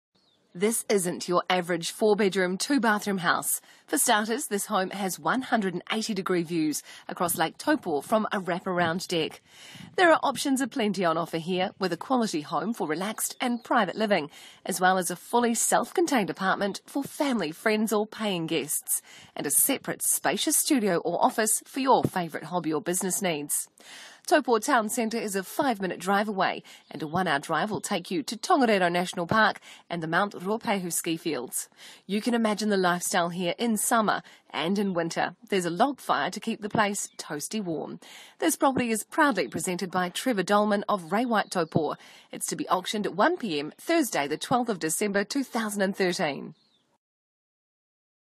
Acacia Bay - Taupo Home & Apartment Supreme - Trevor Dolman - Ray White Taupo
80 Wakeman Road, Acacia Bay
Taupo
Trevor Dolman
Ray White Taupo
80 Wakeman Road
Taupo
Trevor Dolman
Ray White Taupo
Options aplenty on offer here with a quality home with varying entertaining areas for relaxed and private living, a fully self contained apartment for family, friends or paying guests and a separate spacious studio/office for your favourite hobby or business needs. You will also be able to take advantage of substantial renovations and upgrading that has been carried out recently, including a modern stairway chair lift to take you or your Goodies from the garage to the top floor. Located in renowned Acacia Bay, the views of Lake Taupo are panoramic, with walks, park, tennis, boating, dairy and restaurant/bar facilities being all nearby. With Taupo being renowned as the Events Capital of New Zealand you only have a 5 minute drive to Taupo Centre, to participate in or view all the activities that are held every year. This is without doubt a region for the lifestyle purist to enjoy. Act now by phoning me to secure your dream.

To be auctioned at 1.00pm, Thursday 12 December 2013, Ray White Auction Room, 24 Spa Road, Taupo (unless sold prior).